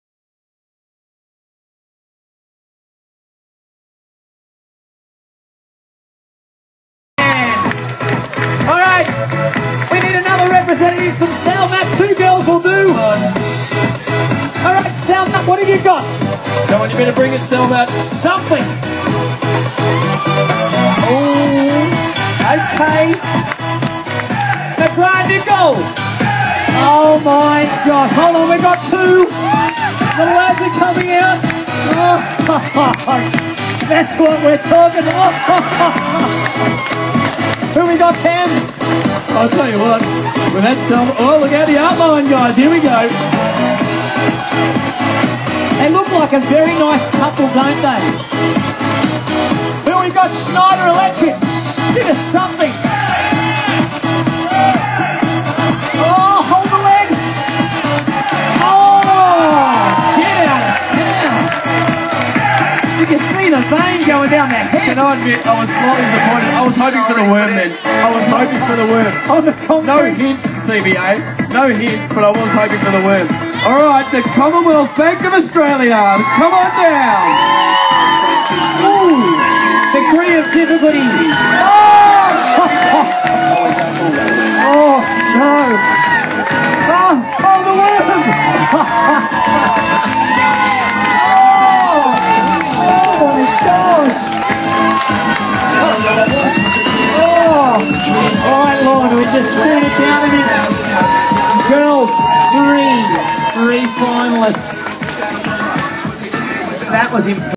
Nissan Corporate Triathlon Sydney 2012
Ther Corporate colours award , where teams strut their stuff and bust out the moves in fron of all competitors.